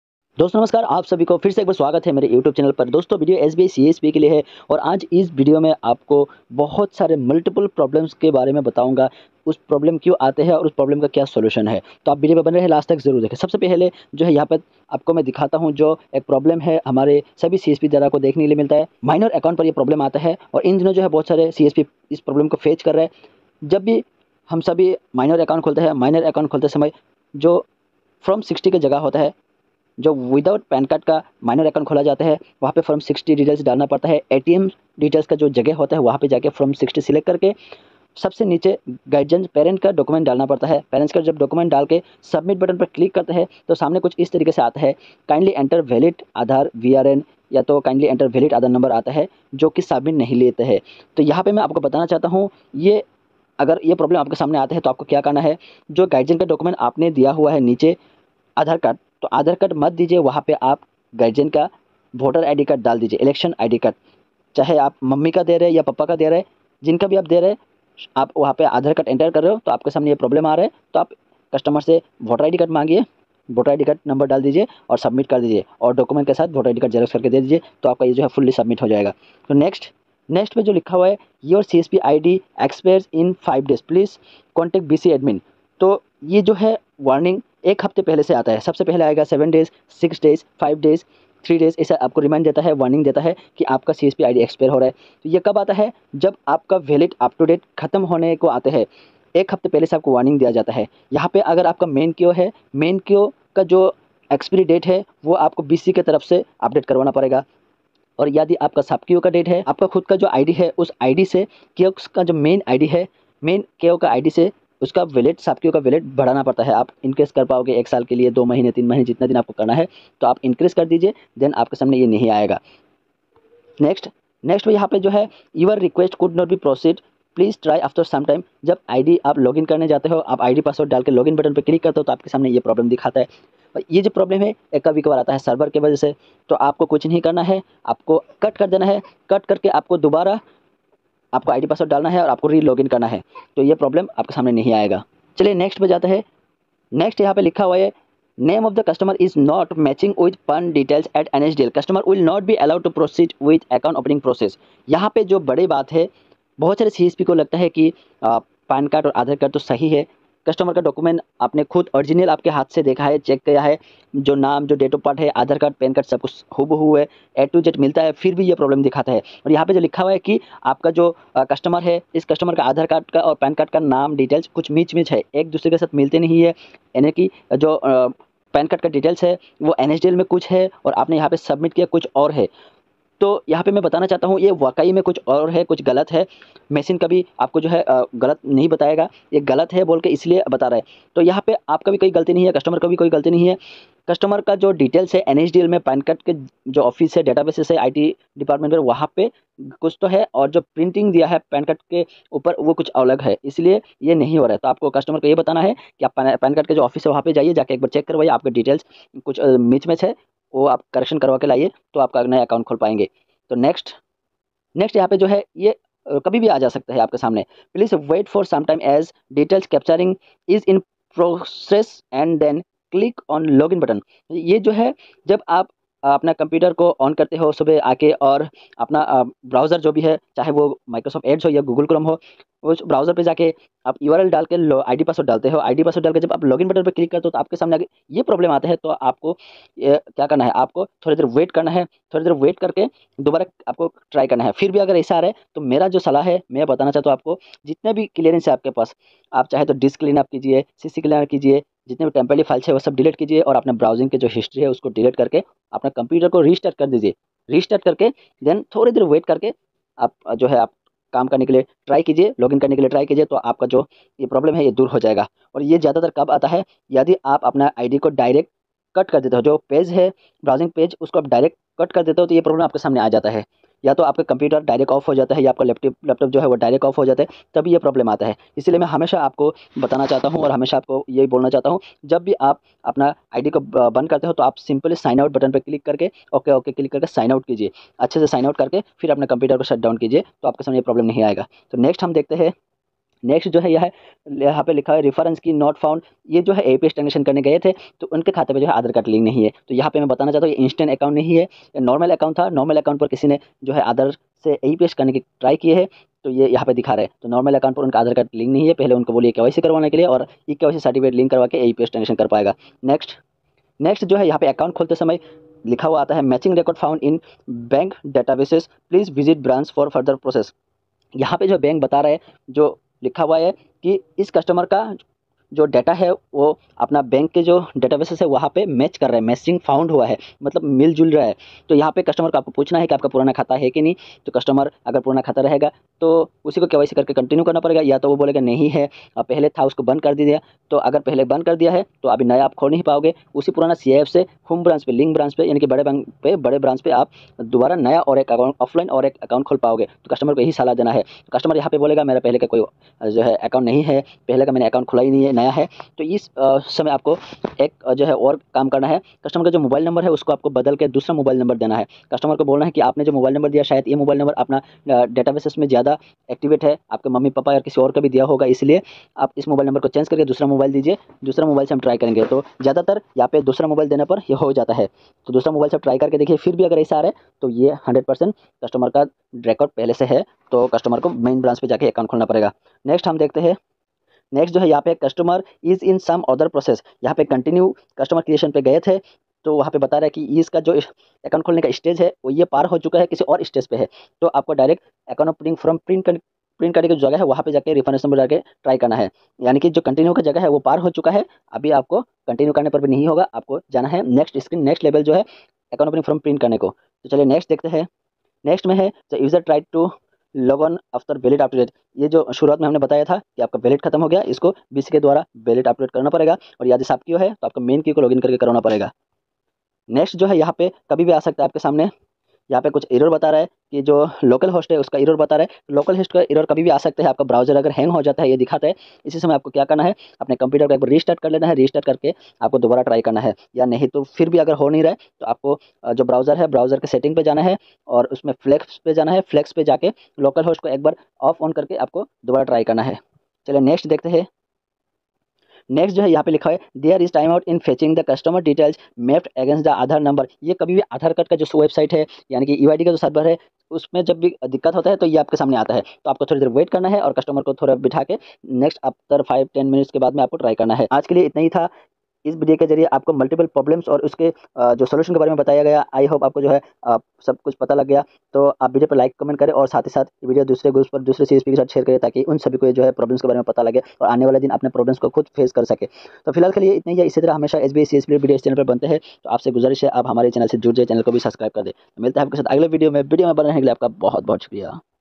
SBI CSP!! इस वीडियो में CSP के अलग-अलग प्रॉब्लम्स और सॉल्यूशन के बारे में बताया गया है !!
इस वीडियो में एसबीआई सीएसपी के अलग-अलग प्रॉब्लम्स और सॉल्यूशन के बारे में बताया गया है।

In this video different problems and solutions of SBI CSP are explained

Mainu Online Tech
Mr Mainu
@Mainu Online Tech

Agar Video achha laga tho